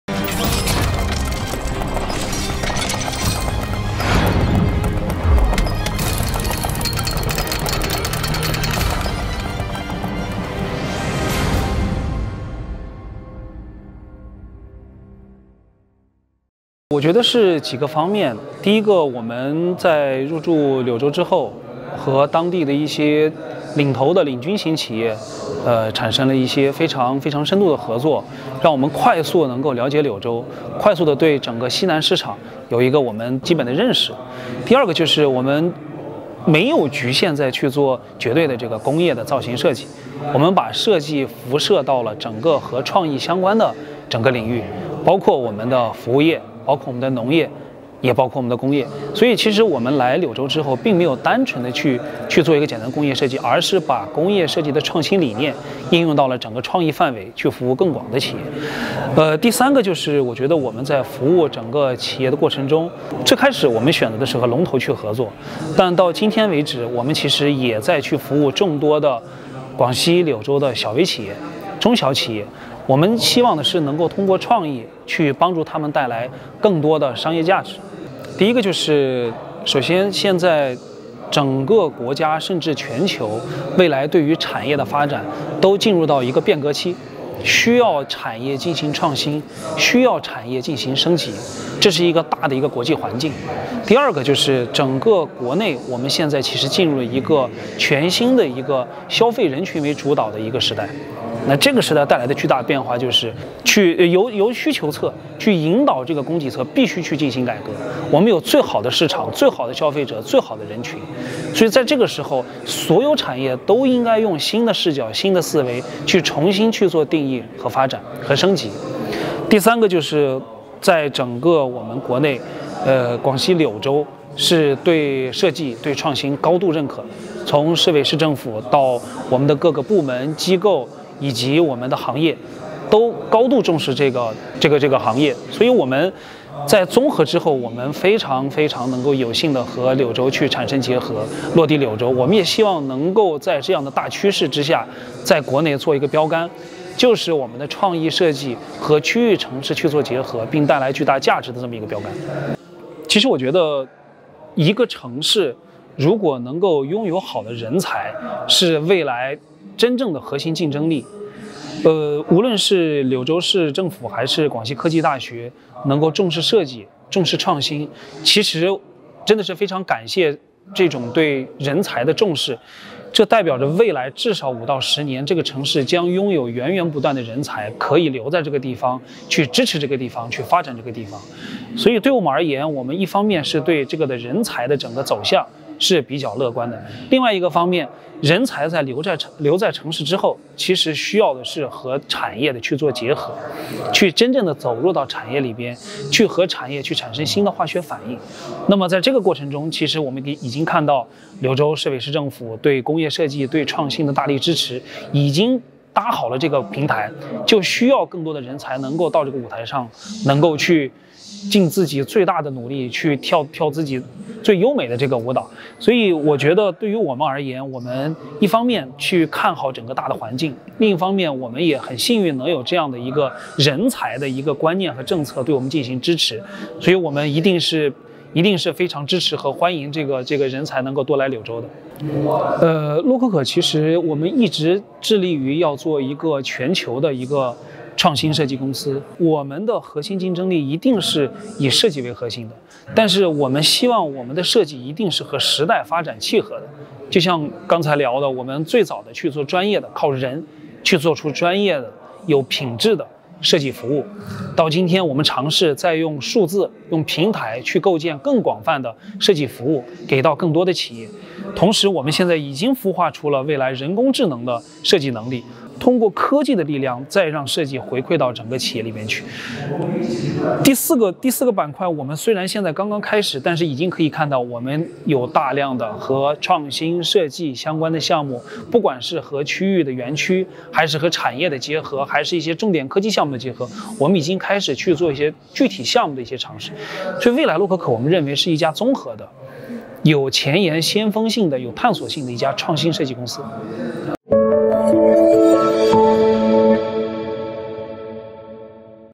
工业设计|第二届中国-东盟工业设计大赛|采访|中国|柳州|工业|创意|Industrial Design|Innovation|Green Economy|2021|Liuzhou|China|蔚江
2021第二届“金紫荆杯”中国-东盟工业设计大赛采访之蔚江篇。
2022第三届即将与你相遇！
热爱工业设计的你，不要错过啦~